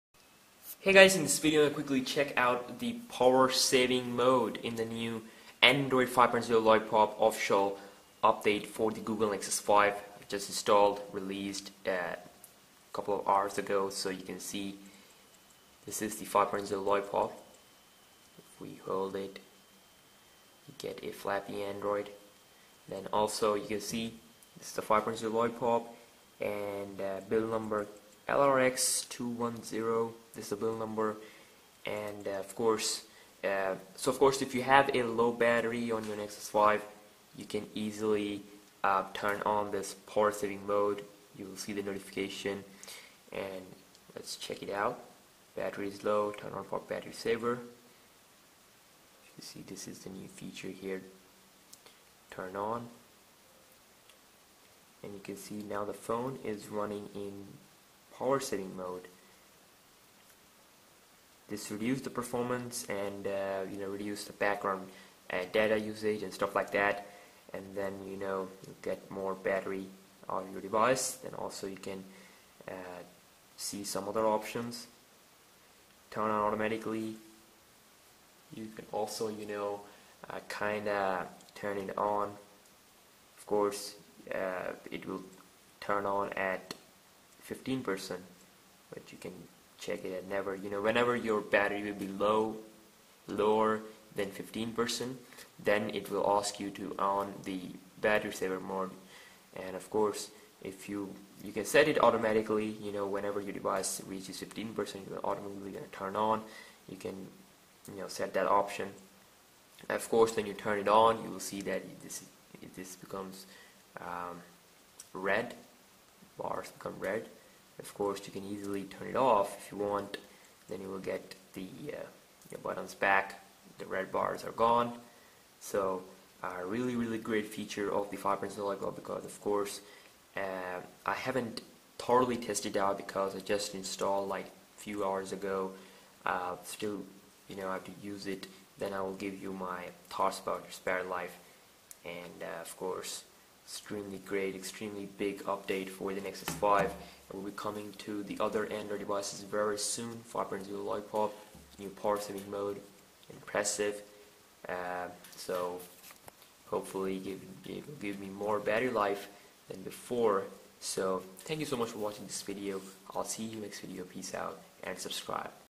NEW Official Android 5.0 Lollipop Battery Saving Mode Review - Nexus 5 HD
Nexus 5 Official Android 5.0 Lollipop New Battery Saving Mode Review.

& Many More!